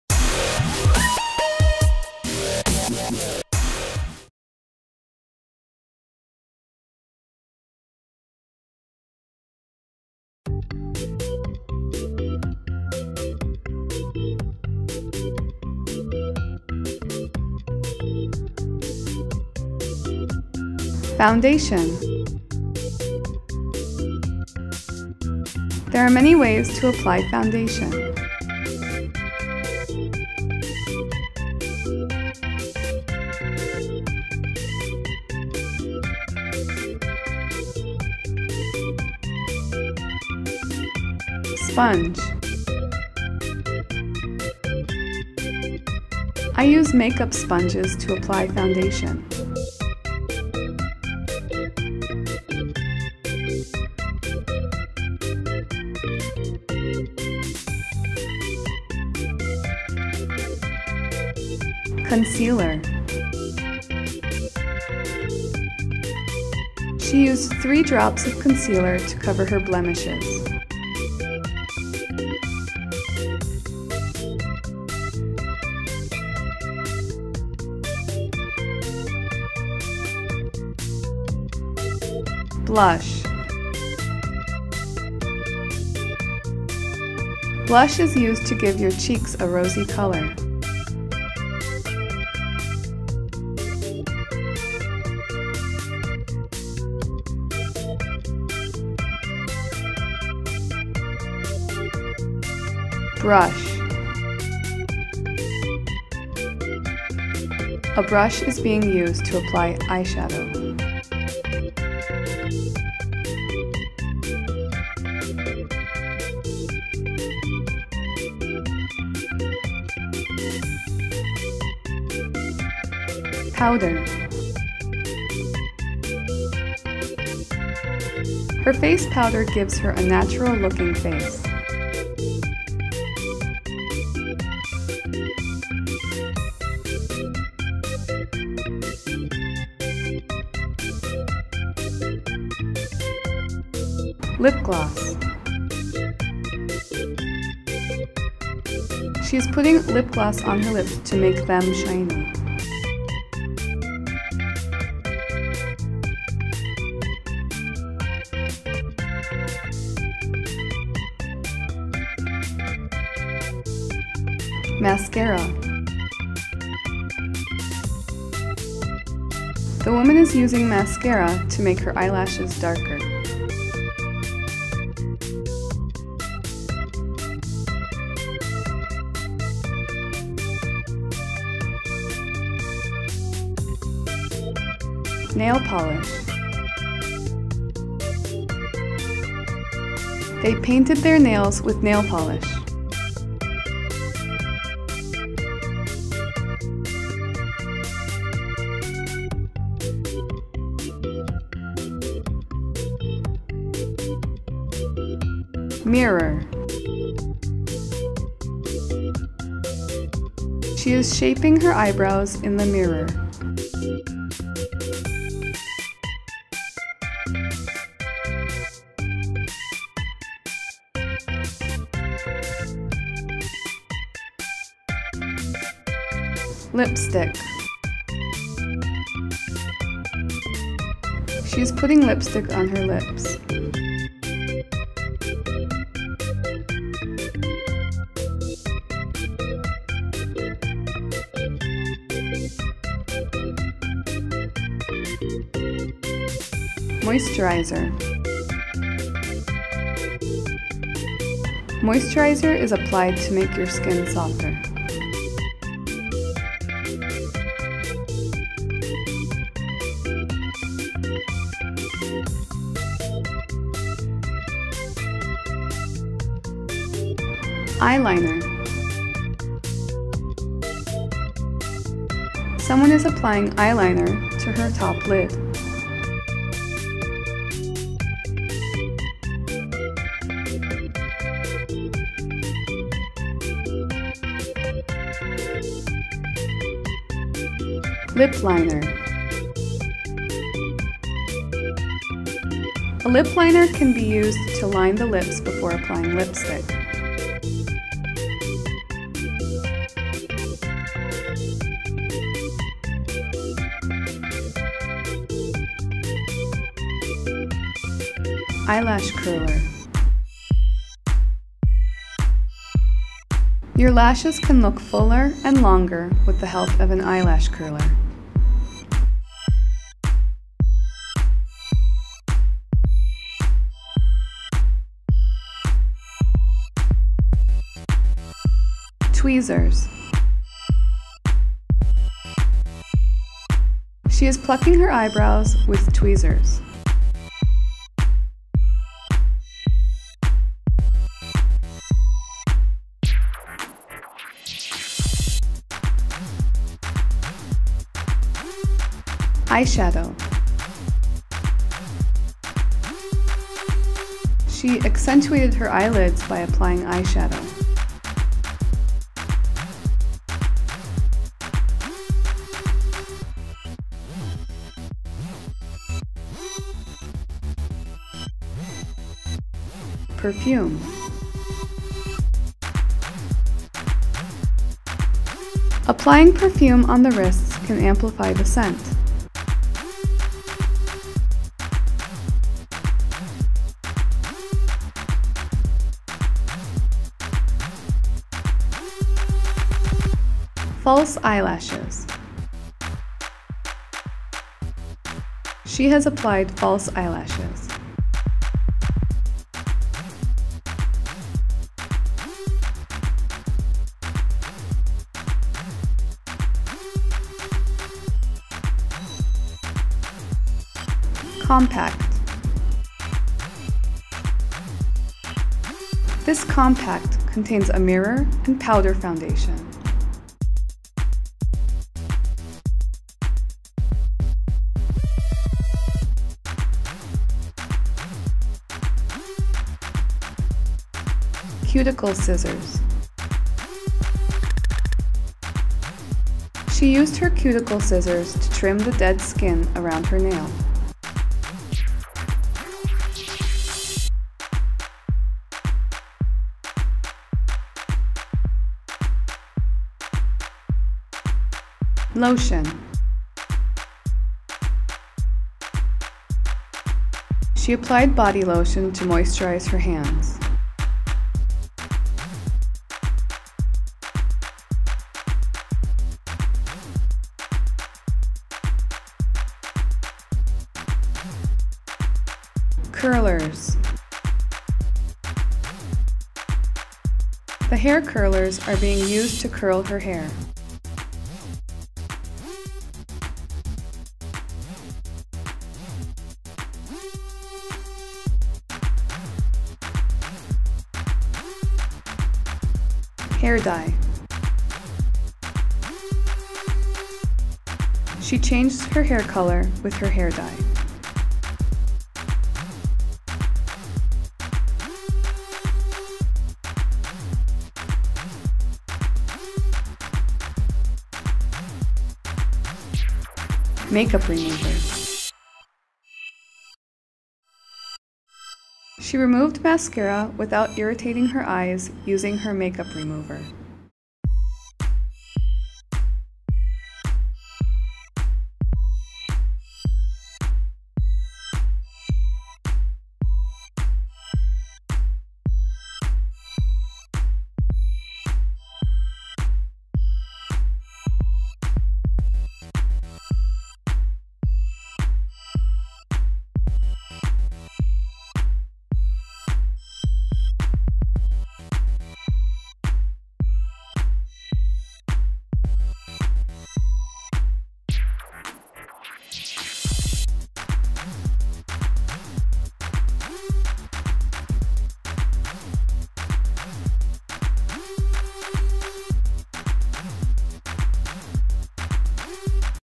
Makeup Tutorial Vocabulary (Part 1: Noun)
Welcome to Dynamic Language Educator!

1. foundation: There are many ways to apply foundation.
2. sponge: I use makeup sponges to apply foundation.
3. concealer: She used 3 drops of concealer to cover her blemishes.
4. blush: Blush is used to give your cheeks a rosy color.
5. brush: A brush is being used to apply eyeshadow.
6. powder: Her face powder gives her a natural looking face.
7. lip gloss: She is putting lip gloss on her lips to make them shiny.
8. mascara: The woman is using mascara to make her eyelashes darker.
9. nail polish: They painted their nails with nail polish.
10. mirror: She is shaping her eyebrows in the mirror.
11. lipstick: She is putting lipstick on her lips.
12. moisturizer: Moisturizer is applied to make your skin softer.
13. eyeliner: Someone is applying eyeliner to her top lid.
14. lip liner: A lip liner can be used to line the lips before applying lipstick.
15. eyelash curler: Your lashes can look fuller and longer with the help of an eyelash curler.
16. tweezers: She is plucking her eyebrows with tweezers.
17. eyeshadow: She accentuated her eyelids by applying eyeshadow.
18. perfume: Applying perfume on the wrists can amplify the scent.
19. false eyelashes: She has applied false eyelashes.
20. compact: This compact contains a mirror and powder foundation.
21. cuticle scissors: She used her cuticle scissors to trim the dead skin around her nail.
22. lotion: She applied body lotion to moisturize her hands.
23. (hair) curler: The hair curlers are being used to curl her hair.
24. hair dye: She changed her hair color with her hair dye.
25. makeup remover: She removed mascara without irritating her eyes using her makeup remover.